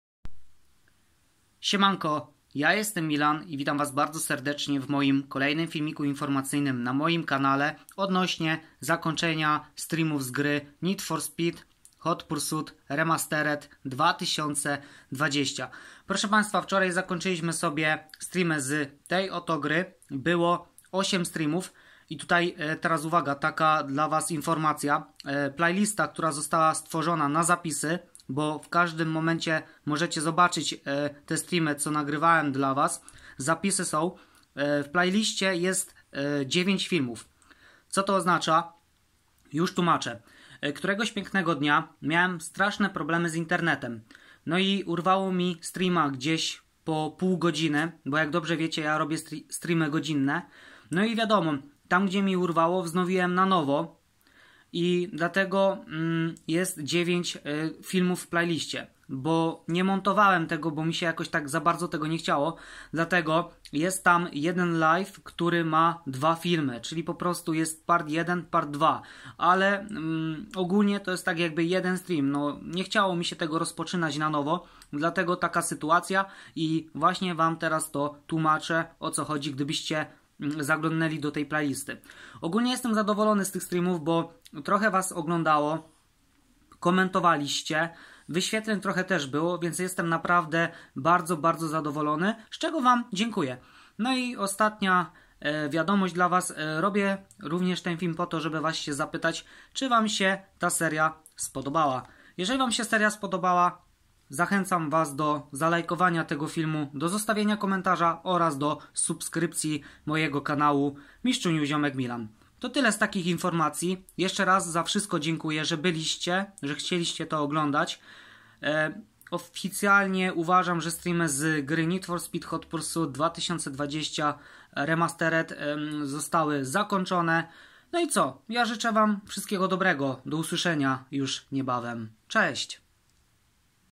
Filmik Informacyjny #156​ Związany Z Need For Speed Hot Pursuit Remastered 2020 STREAM Zakończone :)
Dziękuje jeszcze raz wszystkim którzy byli na tych Stream z tej gry :)